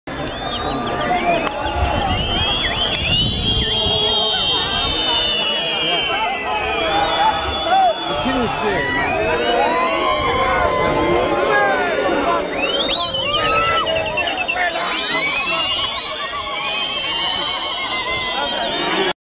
Reed Dance
Annual celebration in Swaziland where the maidens of the country gather reeds to present as a gift to the King. Of these maidens, the King chooses one to be his next wife. A truely cultural African experience.